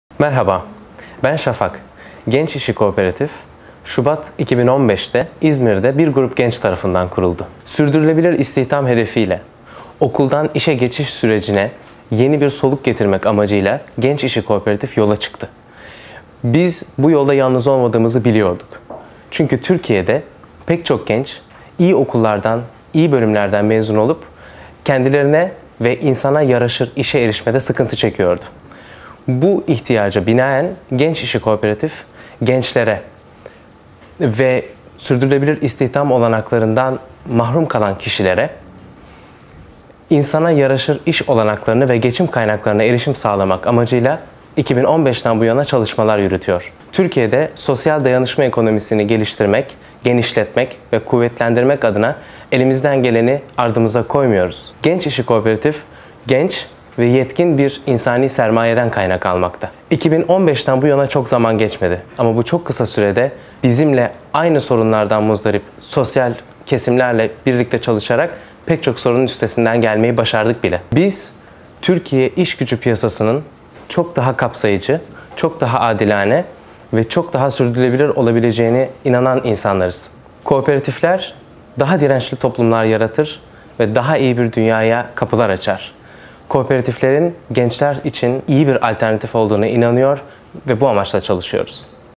Youth Deal Cooperative - Berkin Şafak Şener
Youth Deal Cooperative is Turkey's first youth-led non-profit R&D and training cooperative founded in 2015 in Izmir, Turkey. Youth Deal seeks to improve the access of youth and disadvantaged communities to employment, livelihoods and socio-economic rights.